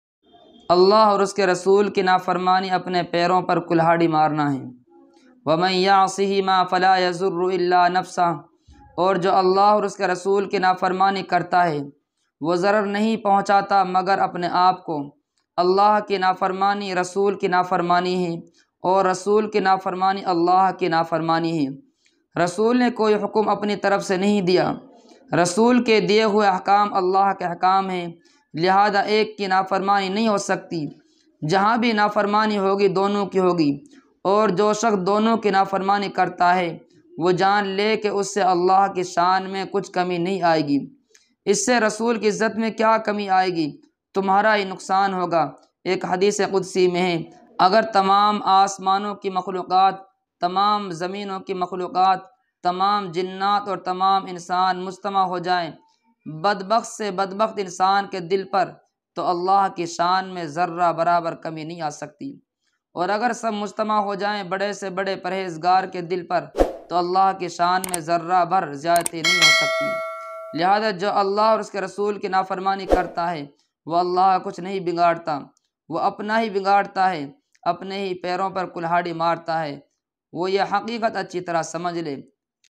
Allah Aur Uske Rasool ki Nafarmani Apne Peron par kulhadi marna hai
Allah aur rasool ki Nafarmani Apne Peron par kulhadi marna hai
Nafarmani mat Karo
اللّٰہ اور اس رسول کی نافرمانی اپنے پیروں پر کلہاڑی مارنا ہے